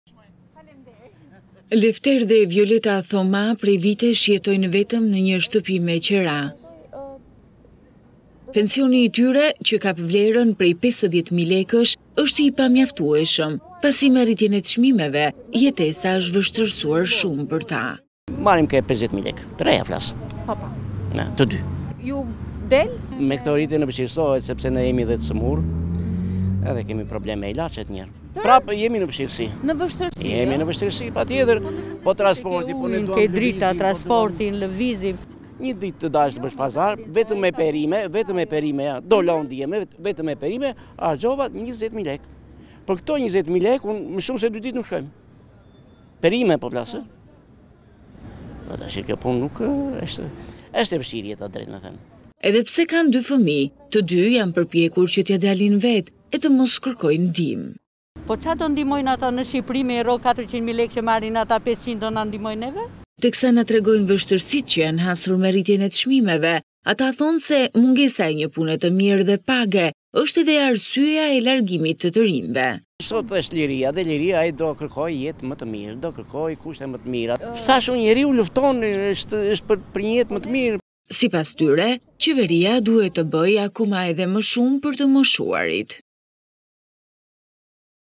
Çifti i të moshuarve: Me shtrenjtimin e çmimeve jetesa na është bërë e vështirë
Pensionistët janë shtresa më e prekur nga rritja e çmimeve. Një çift të moshuarish tregojnë se edhe pse të dy bashkë marrin 50.000 lekë pension me shtrenjtimin e çmimeve jetesa për ta është vështirësuar ndaj apelojnë që shteti t'i ketë në vëmendje.

Çifti i pensionistëve Lefter dhe Violeta Thoma prej vitesh jetojnë vetëm në një shtëpi me qira.

Pensioni i tyre që kap vlerën prej 50.000 mijë lekësh është i pamjaftueshëm, pasi me rritjen e çmimeve jetesa është vështirësuar shumë.

"Marrim 50 mijë të reja të dy. Me këto rritje na vështirësohet sepse ne jemi të sëmurë dhe kemi problem me ilaçet. Sërish jemi në vështirësi", thotë zoti Lefter.

"Ke ujin, dritat, transportin", thotë zonja Violeta.

"Një ditë të dalësh të bësh pazar, dola un dje vetëm perime harxhova 20 mijë lekë. Është e vështirë jeta", thotë zoti Lefter. Edhe pse kanë dy fëmijë, të dy janë përpjekur që t’ia dalin vetë e të mos kërkojnë ndihmë.

"Po çfarë do na ndihmojnë ata me rrogë 400 mijë që marrin në Shqipëri do na ndihmojnë ne?" thotë zonja.

Teksa na tregojnë vështirësitë që janë hasur me rritjen e çmimeve ato thonë se mungesa e një pune të mirë dhe page është arsyeja e largimit të të rinjve.

"Sot është liria dhe ai do kërkojë jetë më të mirën kushte më të mira", thotë zoti Lefter.

Për to qeveria duhet të bëjë akoma më shumë për të moshuarit duke mos i lënë pas dore.